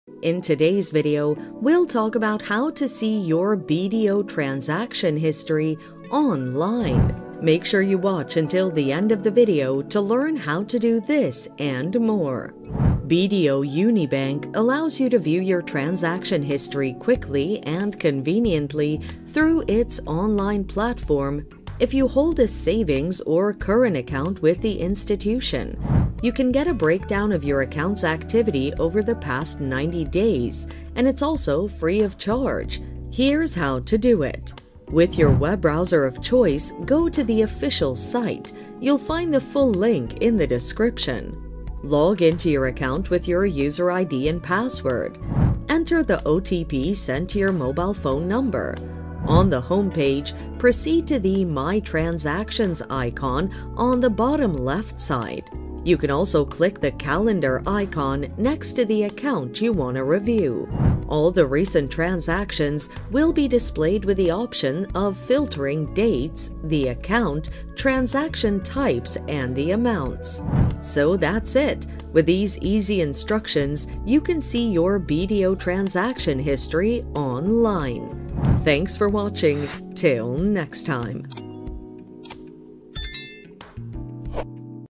How To See Transaction History In BDO Online (How To Check Transactions In BDO Online Banking)
How To See Transaction History In BDO Online (How To Check Transactions In BDO Online Banking). In this video tutorial I will show how to see Transaction History in BDO online.

BDO Unibank allows you to view your transaction history quickly and conveniently through its online platform if you hold a savings or current account with the institution. You can get a breakdown of your account’s activity over the past 90 days, and it is also free of charge. Here is how to do it:
1. With your web browser of choice, go to the official site.
2. Log in to your account with your User ID and password.
3. Enter the OTP sent to your mobile phone number.
4. On the homepage, proceed to the ‘My Transactions’ icon on the bottom left side. You can also click the calendar icon next to the account you want to review.
5. All the recent transactions will be displayed with the option of filtering dates, the account, transaction types, and amounts.